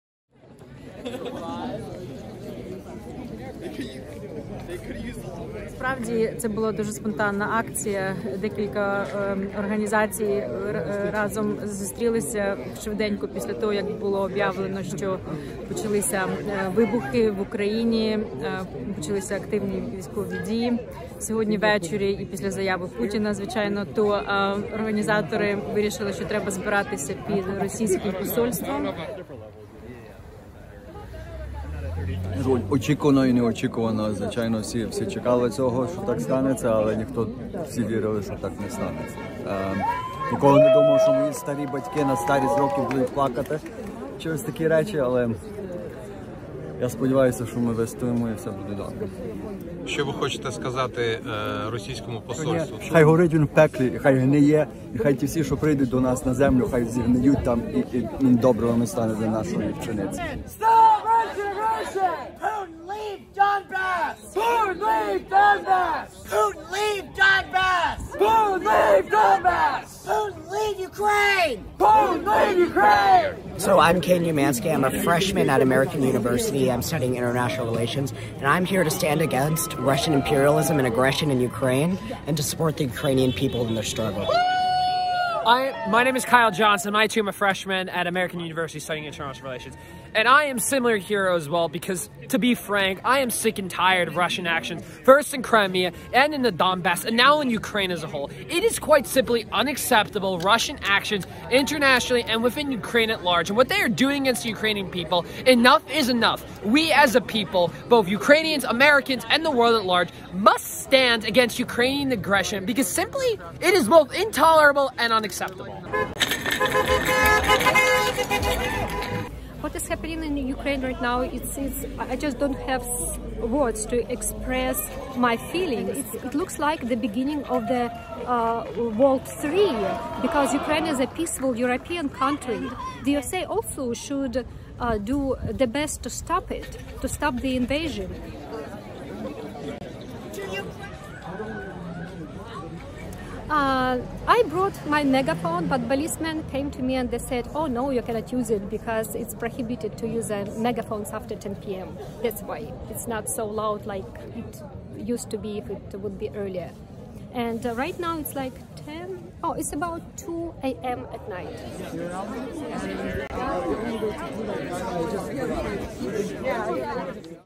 Посольство Росії пікетували в Вашингтоні
У ніч проти 24 лютого, коли Росія вчинила акт збройної агресії проти України, під посольством РФ у Вашингтоні зібралася акція протесту